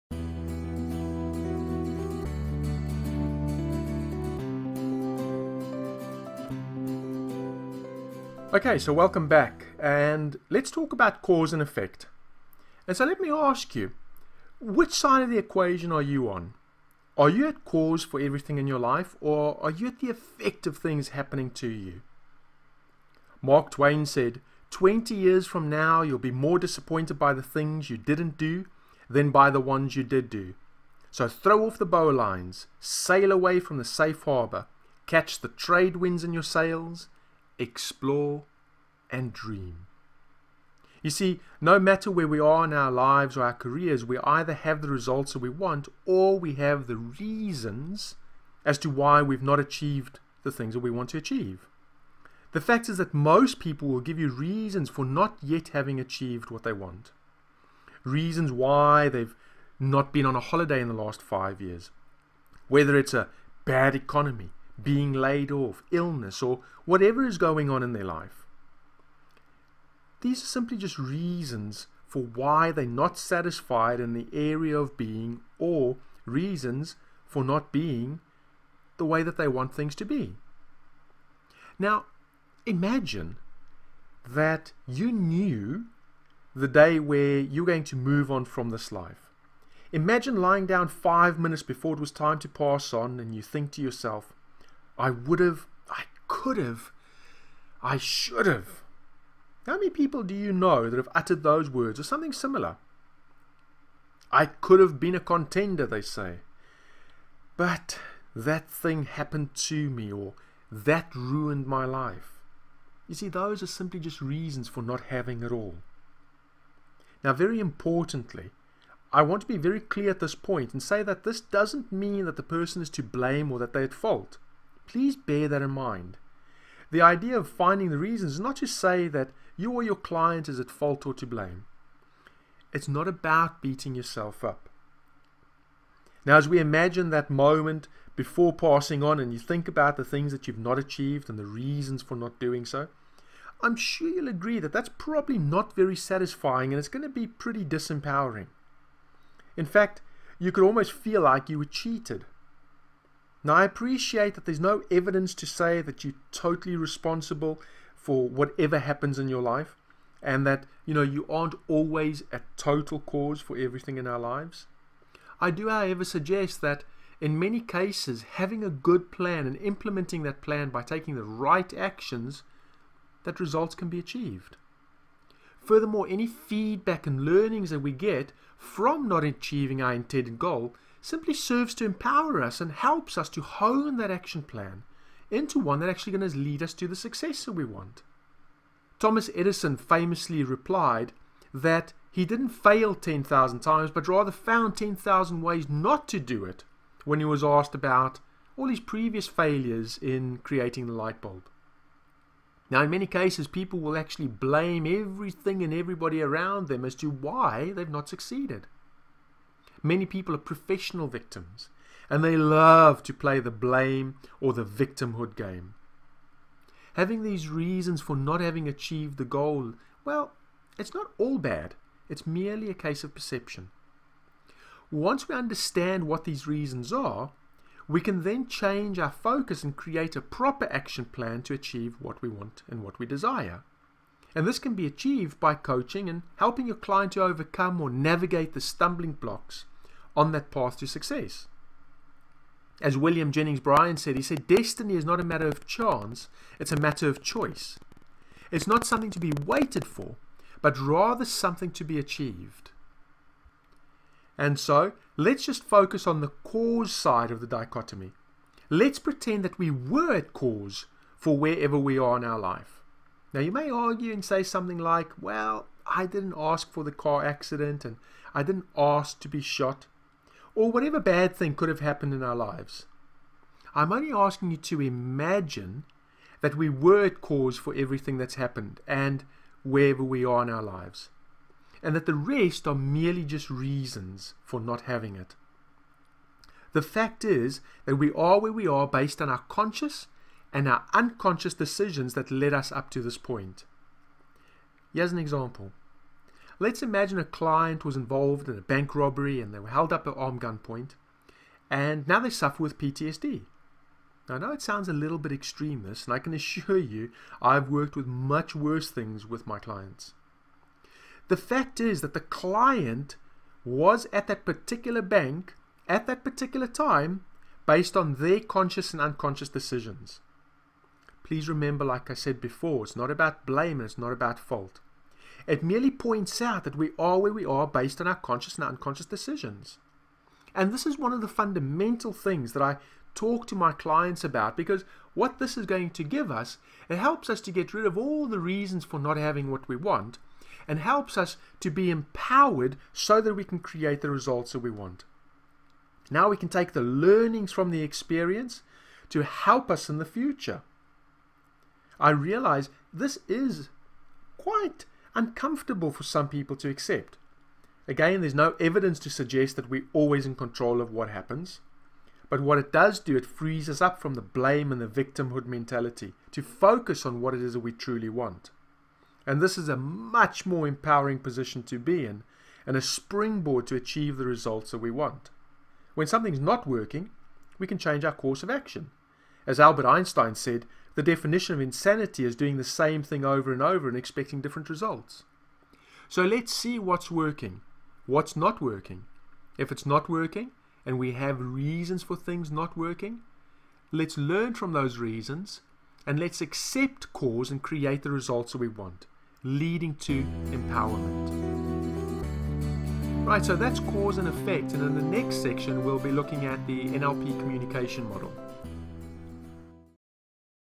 Free NLP Coaching course. Video 3- Cause and Effect
This is video 3 in the free NLP coaching course.

In this video we look at Cause and Effect and what implications it has in our life.

We either have the results we want in life, or we have reasons why we do not. The thought is that if we can accept that we are at cause for were we are in our life, then we can create the life we want.

To watch all the video in this series, download the audios or manual, simply

If you would like to take the full online NLP practitioner and life coaching course, then you can do so